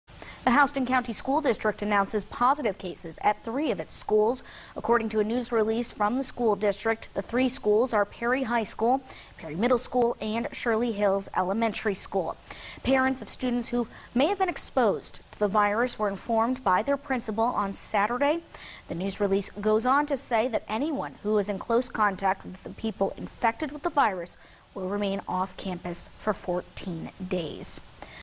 Houston County School District confirms 3 positive COVID-19 cases
The Houston County School District confirmed its notification of positive COVID-19 cases at three of its schools.